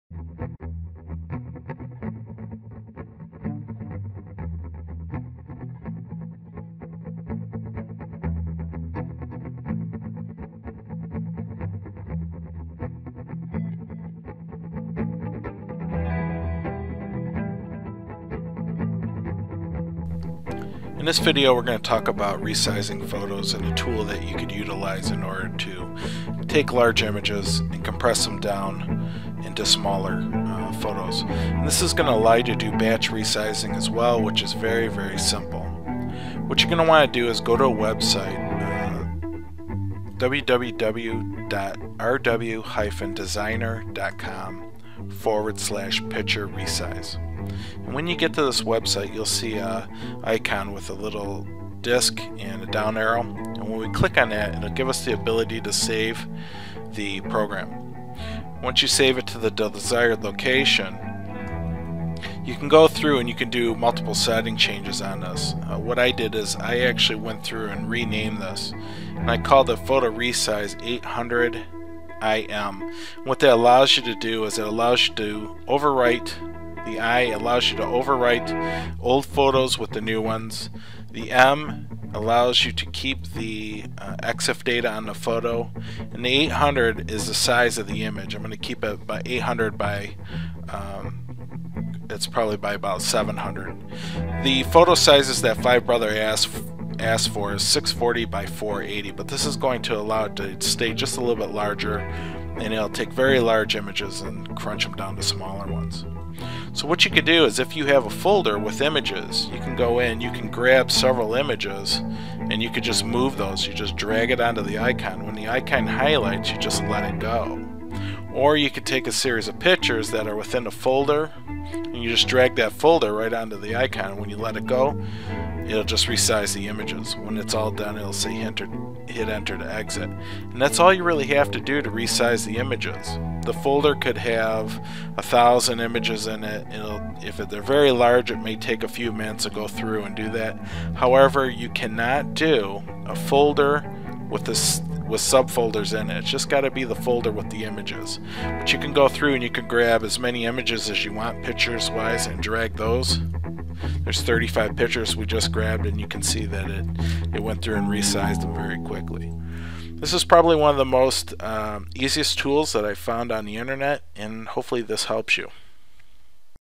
Five Brothers - Photo Resize Tool Suggestion
This is a helpful video on baatch resizeing photos to smaller sizes. You can use the link below to link to the product described in this video. This is one of the fastest and easiest tools found on the internet. Its free and very simple to use.